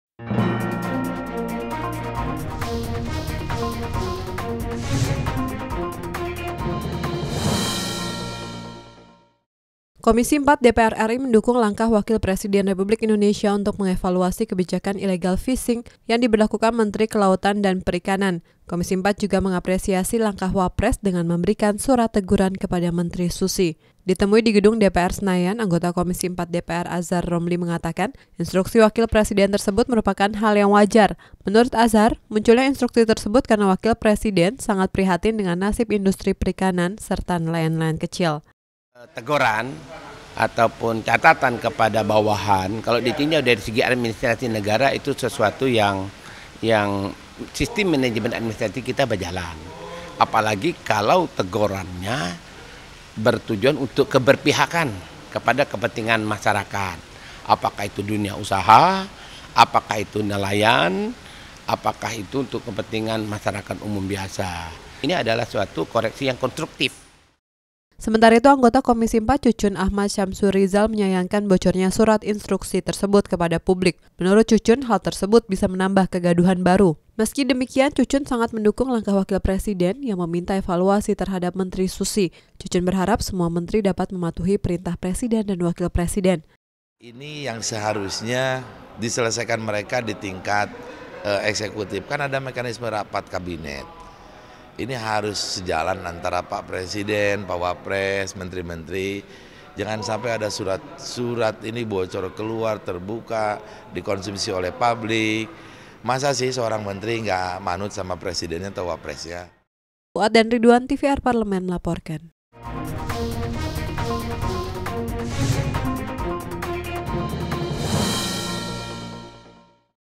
DPR RI - KOMISI IV DUKUNG EVALUASI KEBIJAKAN ILLEGAL FISHING
NARSUM : AZHAR ROMLI / Anggota Komisi IV DPR RI / FPG / Dapil BANGKA BELITUNG
CUCU AHMAD SYAMSURIJAL / Anggota Komisi IV DPR RI / FPKB / Dapil JABAR II
ANDI NAWIR / Anggota komisi IV DPR RI / FPGERINDRA / Dapil SULSEL III
(RTV Parlemen - Biro Pemberitaan Parlemen - SETJEN DPR RI)
FB : DPR RI & RTV Parlemen DPR RI
Youtube : DPR RI & RTV Parlemen
(IKI)